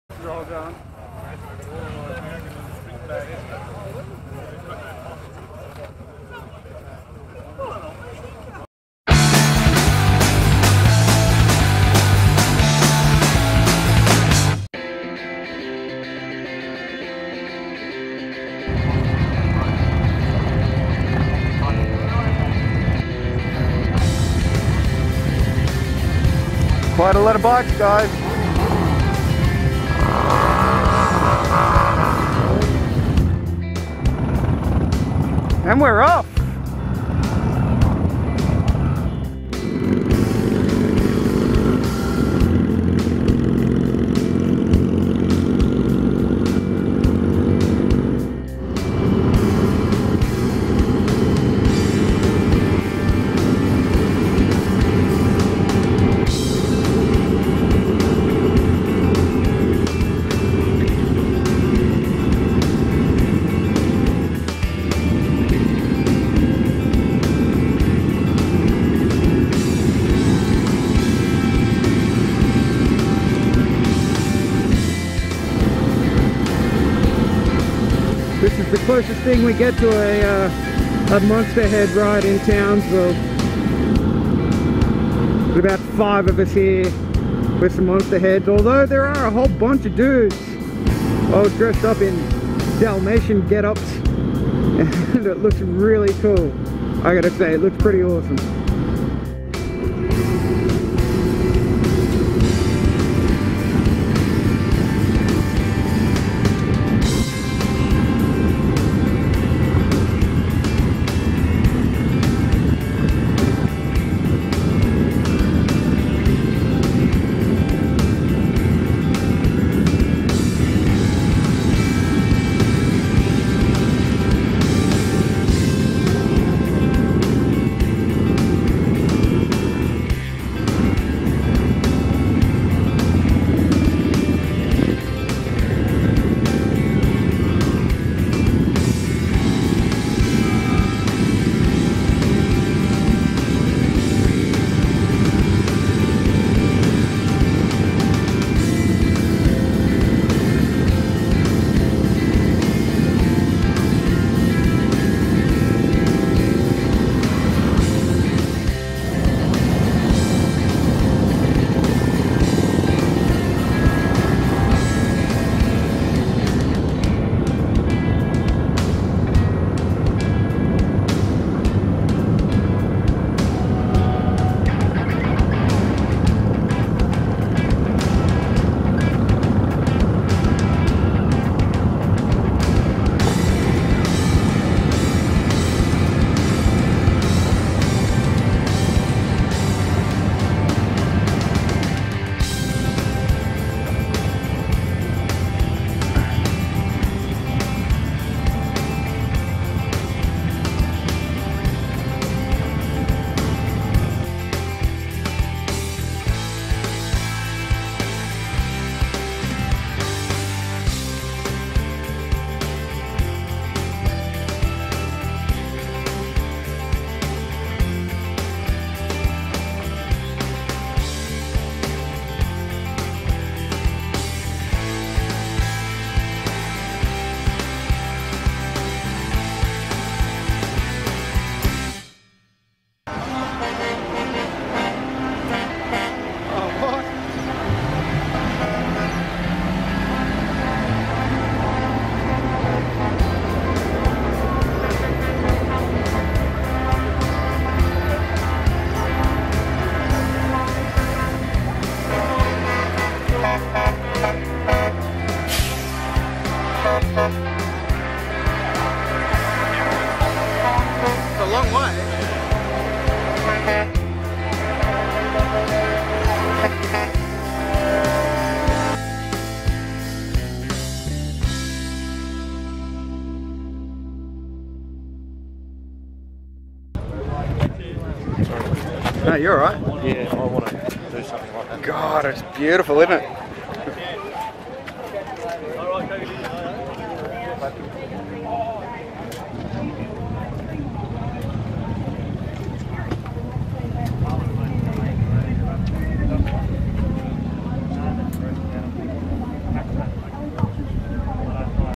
Townsville Camp Quality Convoy
Camp Quality is a charity organisation that helps children with cancer get away from the day to day struggle of getting better and gives them an opportunity to get out and have some fun, the chance to be a kid!!!   For more information on this fantastic organisation click this

There isn't much talking in this as I don't like group rides very much and I need to concentrate to counter the other riders I don't know.

This ride was the 2015 Camp Quality Convoy in Townsville. The event had roughly 300 Trucks and 250 Motorcycles of all shapes and sizes. of course mine was the best though :-)

Riding a 2007 DL650 V-Strom.
Camera - Drift Ghost HD, GoPro Hero 3 Black, SJ4000